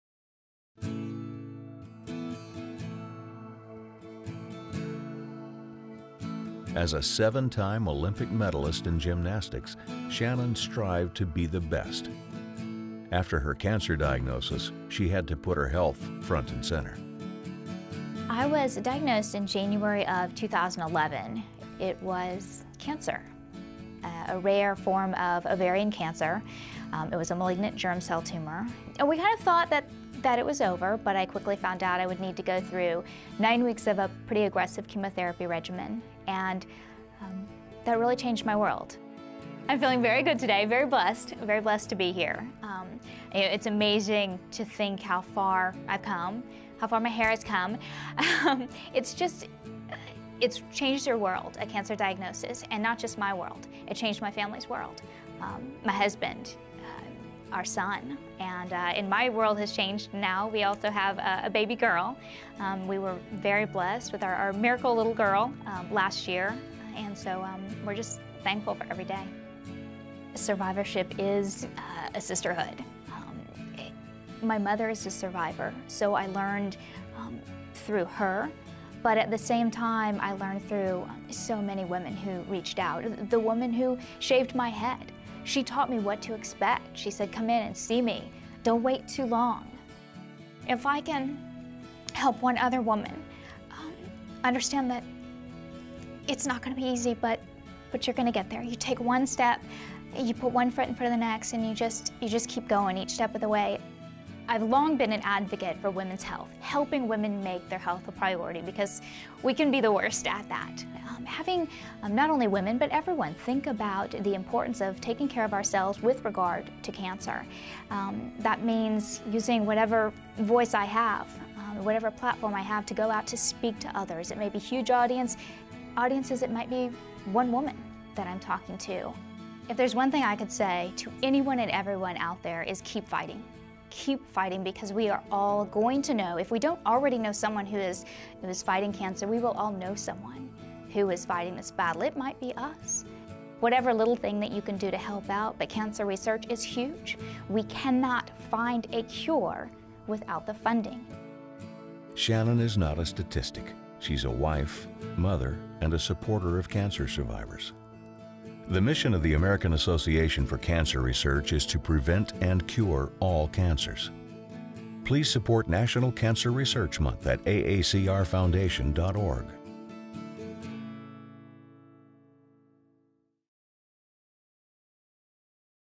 She's Not a Statistic: Shannon Miller's Story
More than 1.6 million people in the United States will be diagnosed with some form of cancer in 2014. Seven-time Olympic medalist, mother, and ovarian cancer survivor Shannon Miller is one of them. This is her cancer story.

Cancer research saves lives. Please support cancer research during May, National Cancer Research Month, and throughout the year. to find out how you can help.

The mission of the AACR is to prevent and cure all cancers.